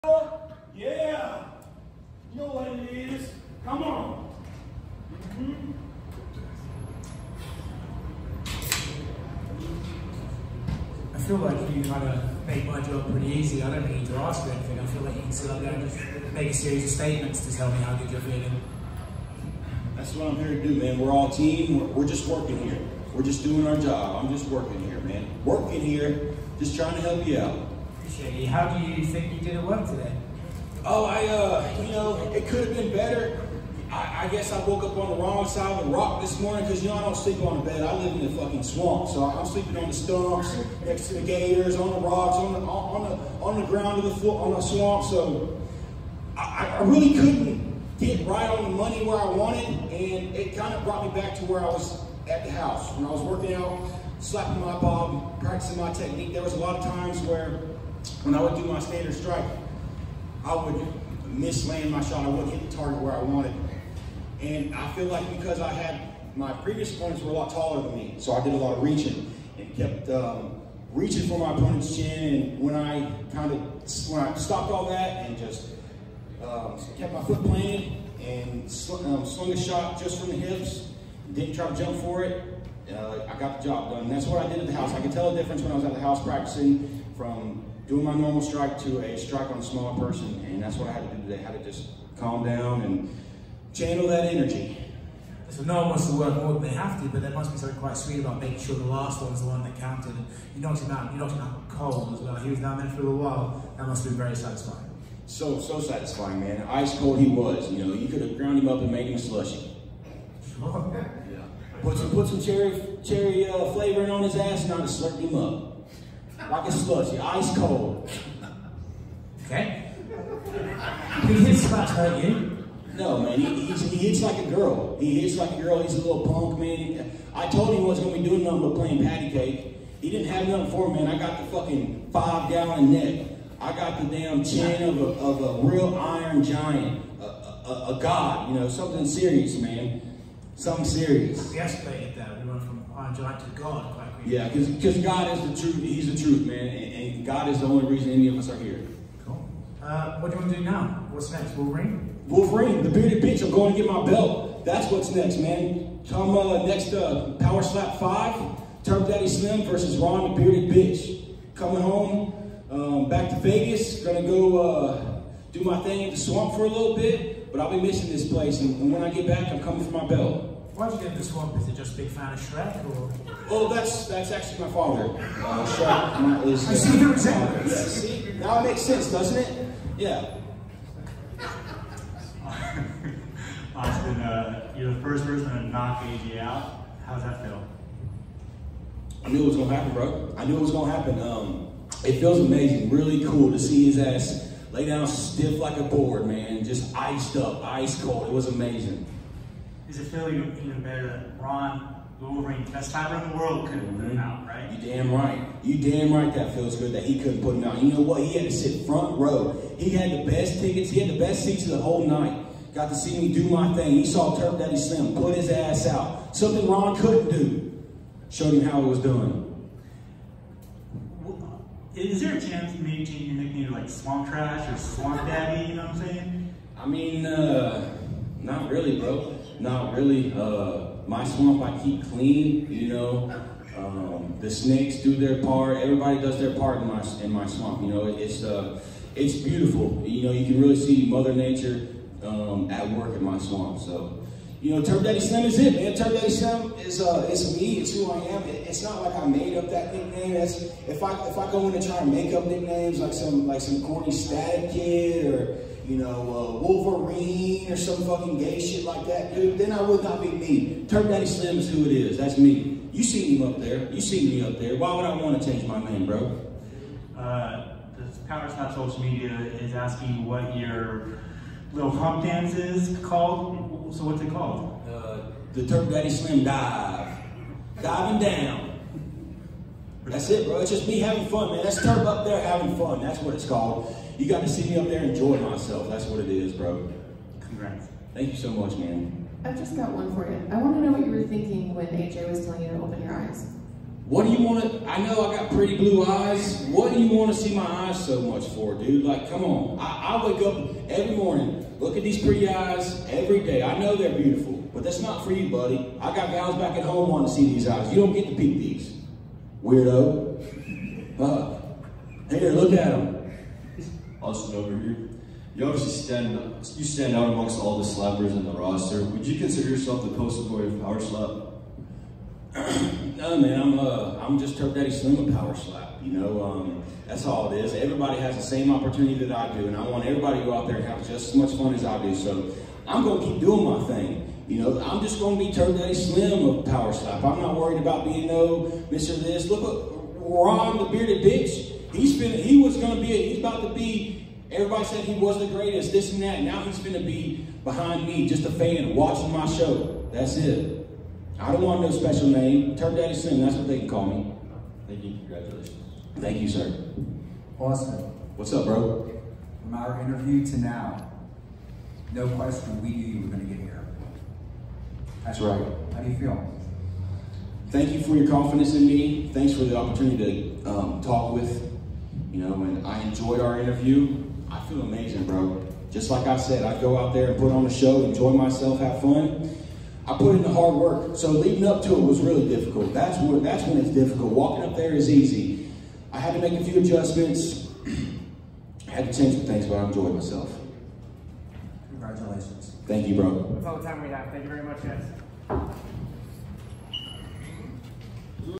Austin Turpin, I don’t sleep on a bed. I sleep in the swamp on rocks. Power Slap’s true madman.
Power Slap post fight press conference and full interview after Austin Turpin KO ‘d AyJay Hintz.. Austin did his “Slap Worm” as we also witnessed just one back at Power Slap 3.

Austin brought it on after knocking out AyJay in Power Slap 4.

After the knockout and giving the fans a exciting celebration of his winning he stepped into the media area of UFC Apex.

Stepping up to the stage and before hand also Austin was shouting and arms waving. True madman.

With this noise and action going on before he steps foot onstage you know you are in for a good one.

Austin delivered. As I also witnessed at Slap 3 Austin tore threw the questions. He gave some unexpected answers, thoughts, views on his opponent, glances into home life back in Jacksonville, Florida, snakes, alligators, why do you need to see my eyes?, calling (AyJay) weirdo.

All statements and comments are paraphrase￼

Craziest,Strangest, funniest  Power Slap interview to date. Can’t wait to see what he does next,